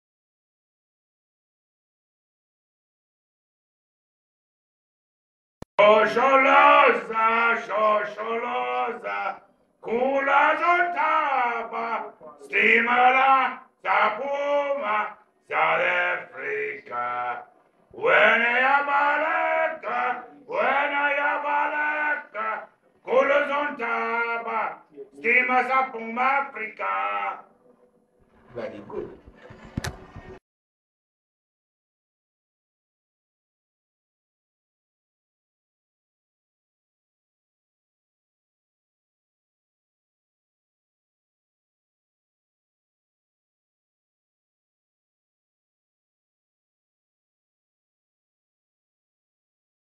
Graham New
Graham being tone deaf doing his absolute best to win a shot at a free ticket to the Rugby World Cup 2011 in New Zealand. WHAT A BEAUT!